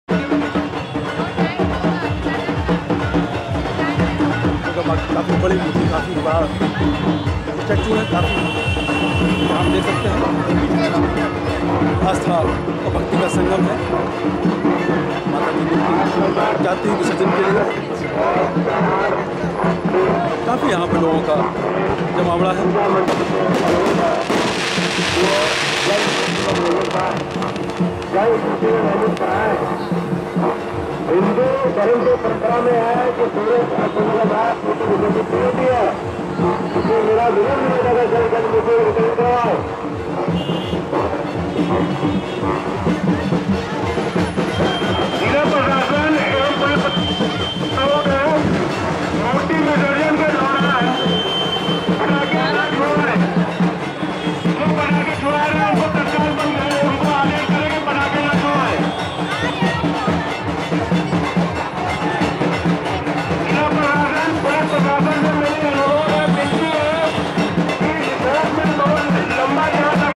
Mata Durga`s Murti Visarjan at Gomti River
This Documentary is based on Mata Durga`s Murti Visarjan .it is by Mystery History.